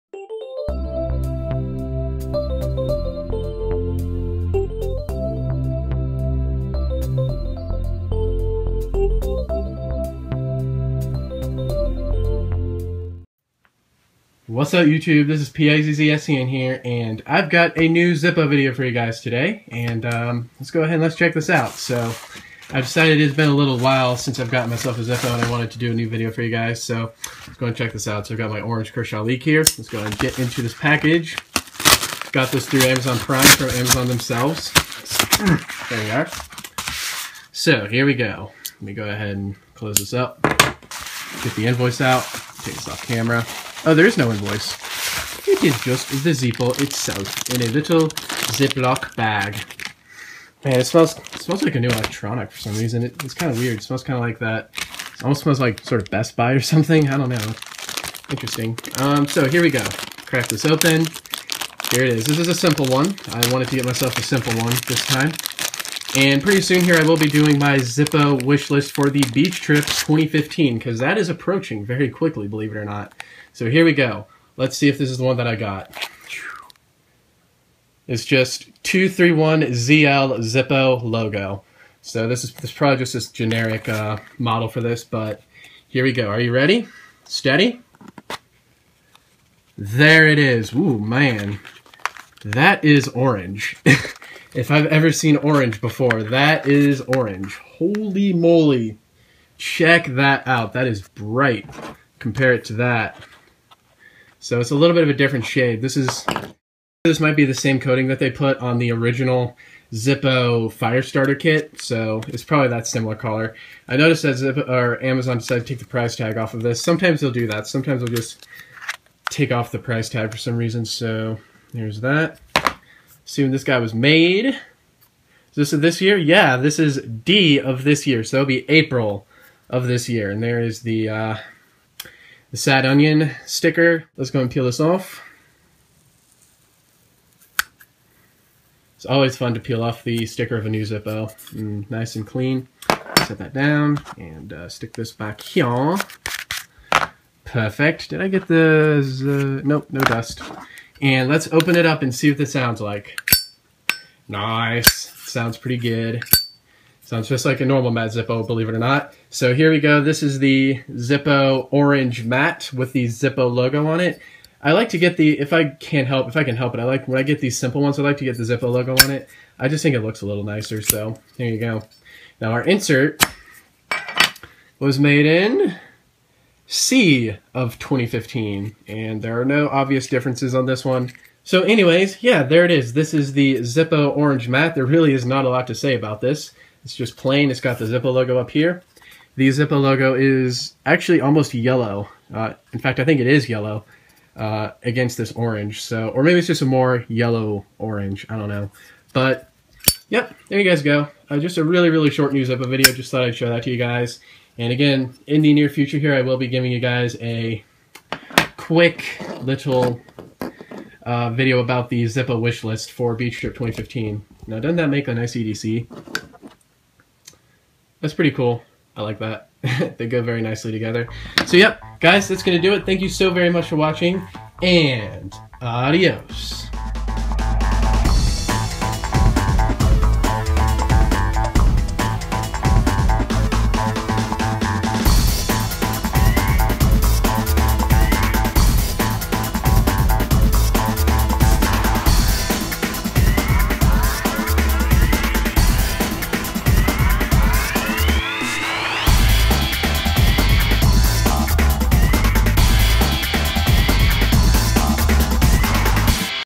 New Zippo - Orange Matte w/ Zippo Logo (231ZL)
Finally, a new Zippo video!  This time it's orange :)

There is a small issue I've been having with my editing software, Screenflow, that has been making it harder for me to produce videos.  You'll hear towards the beginning of this video a silent part.  Lately my vids have been exporting with lots of long silent parts in them, making the video unwatchable.  Please bear with me guys.  I have another video coming soon, and once I can figure out this bug, I'll post that one ASAP :)

LINKS
But your own: